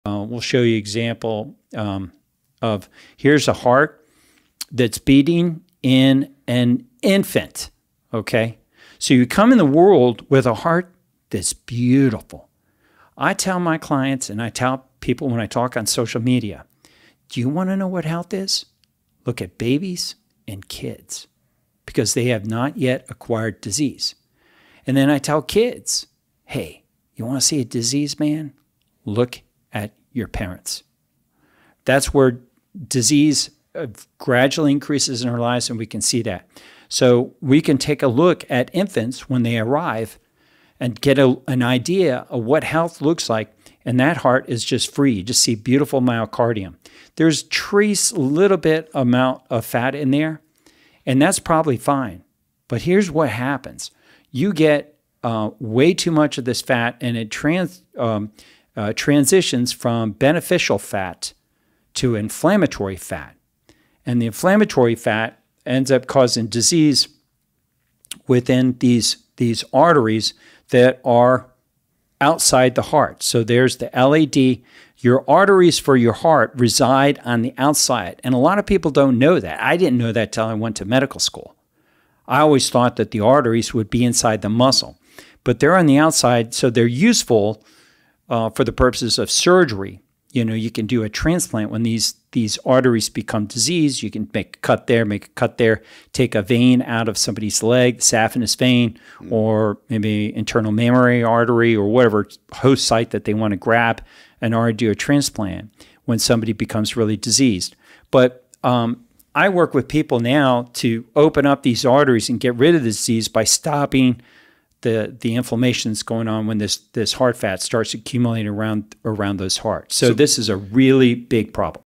Healthy Heart VS Diseased Heart: See the Amazing Difference!
See a healthy infant heart vs. a diseased adult heart. How fat accumulates and causes inflammation, leading to heart disease. Discover ways to reverse heart disease by reducing inflammation. Is it possible to prevent heart disease?